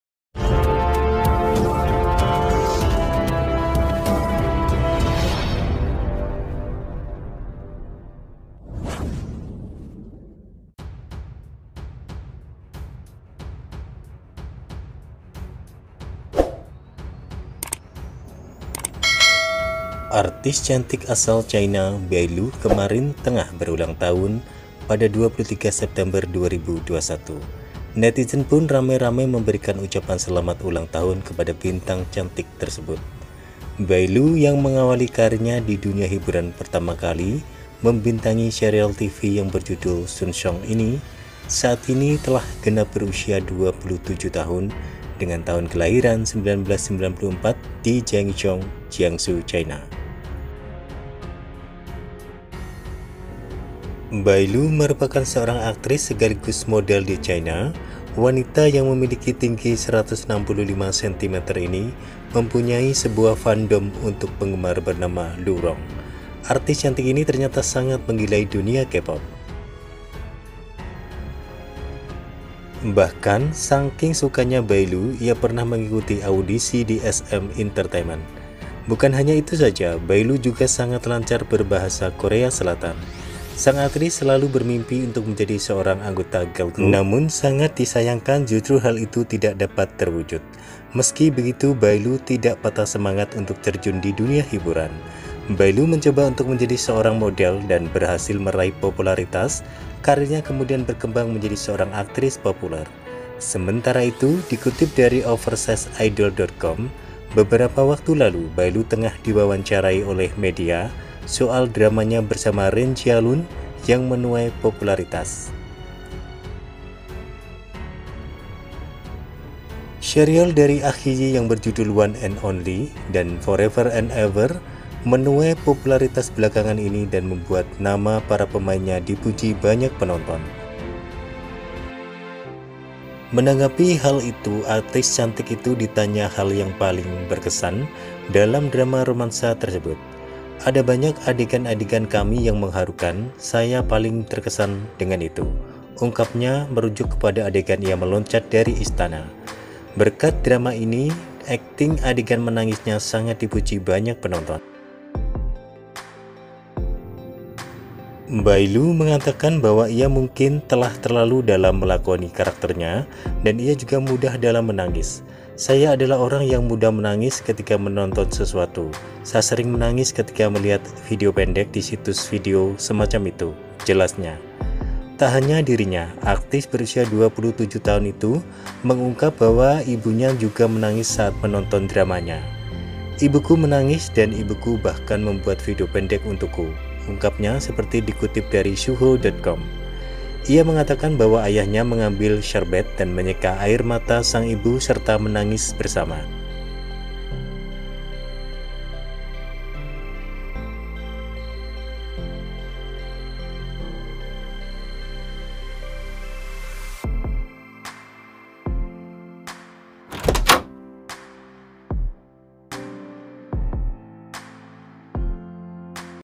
ULTAH BAI LU KE 27 TAHUN " UNGKAP " PERNAH DI " TOLAK " SM ENTERTAINMENT ❓
berita terbaru artis china